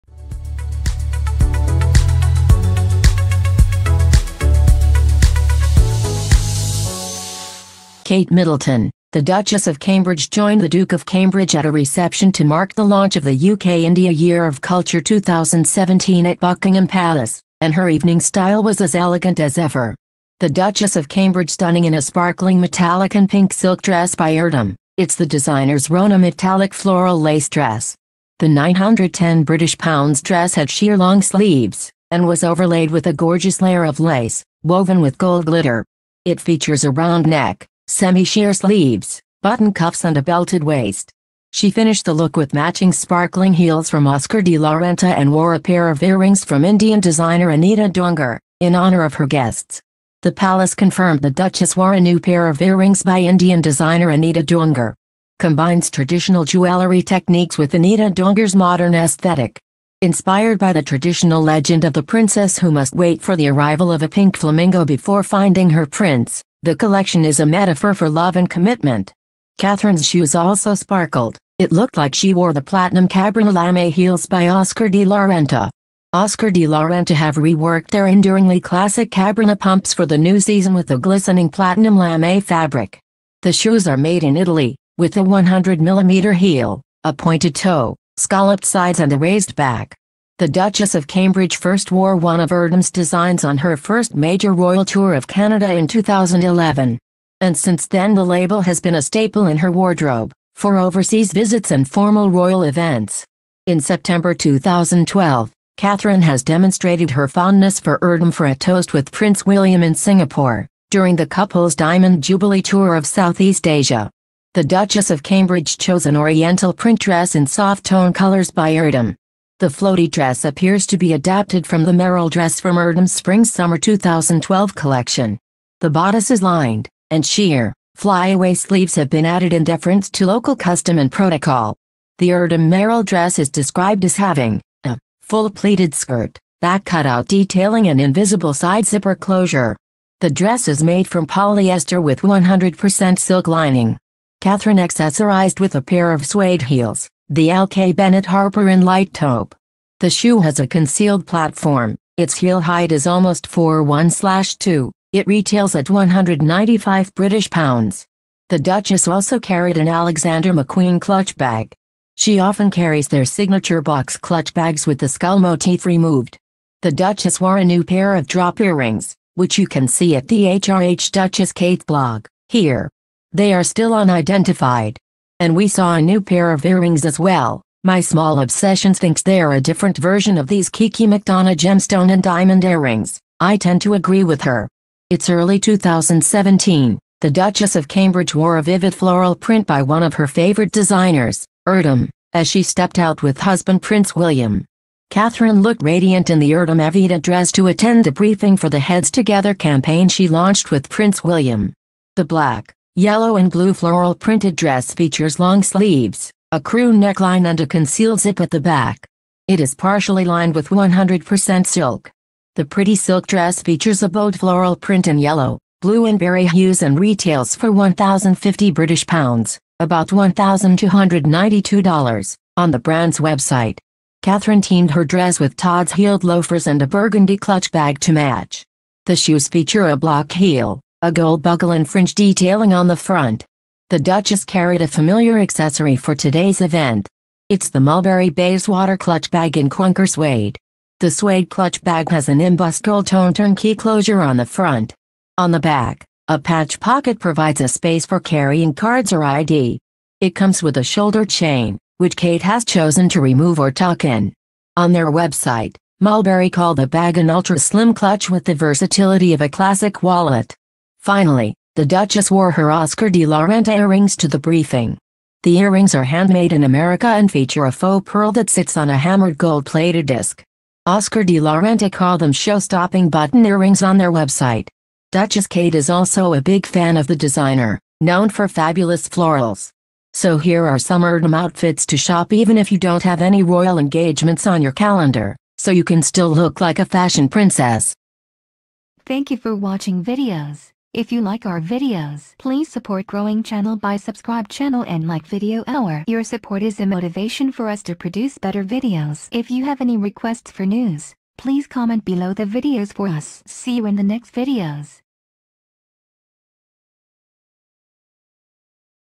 Kate Middleton Stunning In Glittering Dress By Erdem Attends Uk India Year Of Culture Launch
Kate Middleton Stunning In Glittering Dress By Erdem Attends Uk India Year Of Culture Launch/ NPV New

👉 Kate Middleton Dazzles Wore A Long Red Sequin Gown By Needle & Thread For The Evening Reception